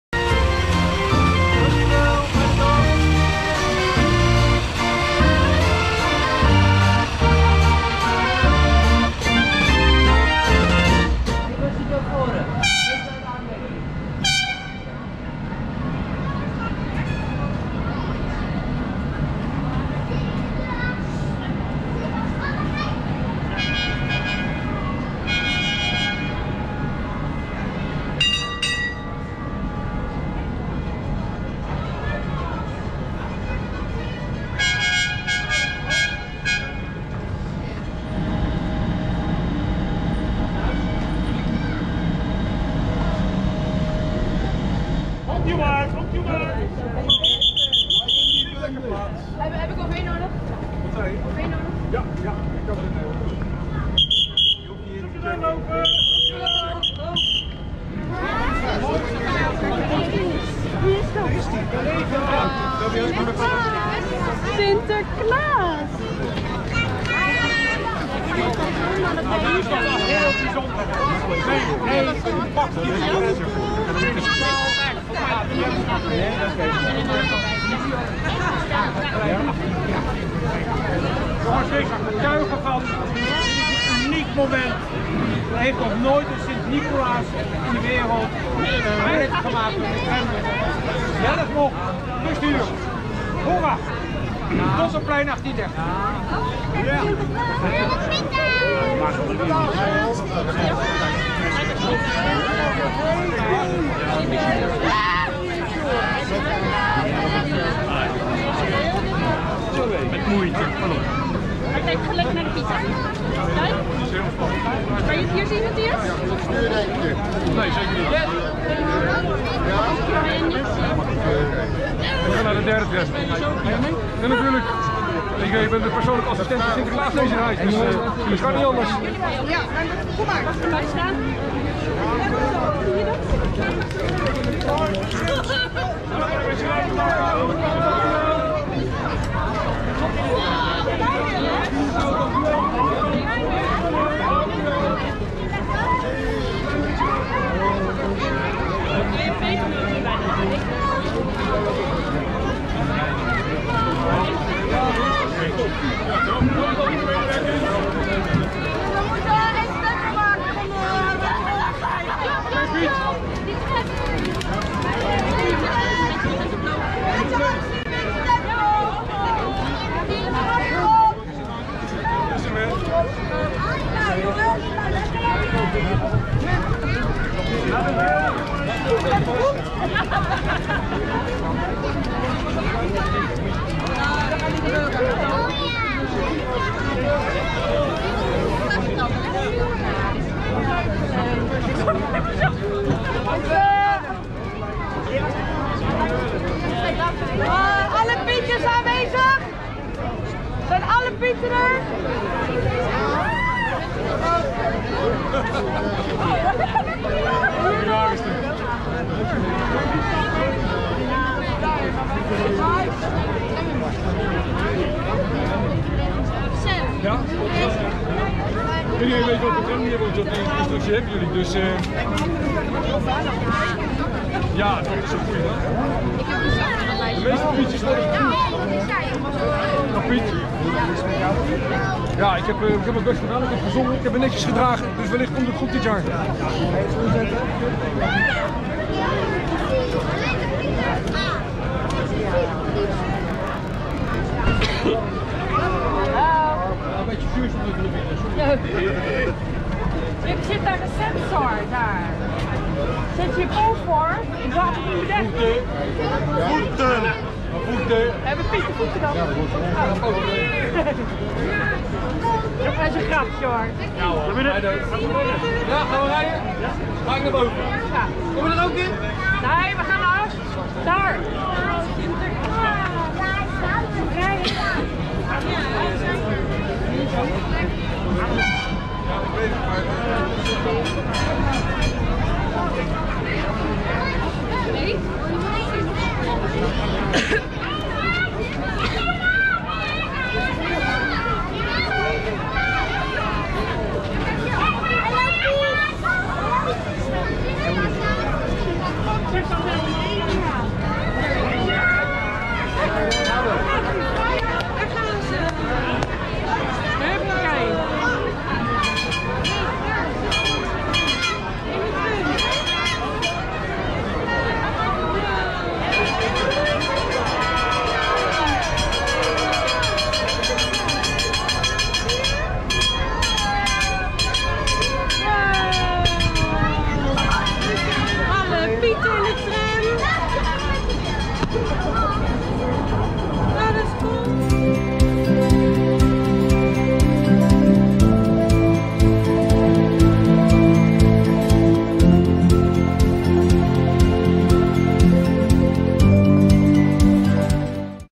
SINTERKLAAS rijdt de PAKJESTRAM: Intocht Sint Nicolaas Den Haag 2022
Zie ginds komt de Pakjestram! Tijdens de intocht van Scheveningen naar Den Haag reden de Sint en zijn Pieten vanaf de Fahrenheitstraat met de Pakjestrams. Sinterklaas bestuurde Pakjestram 1 zelf!
During His tour from Scheveningen to The Hague, Sinterklaas and his helpers made use of special trams. Sinterklaas himself was tramdriver of Pakjestram!!!
Foto Thumbnail: haagstramnieuws.org (veel dank / many thanks).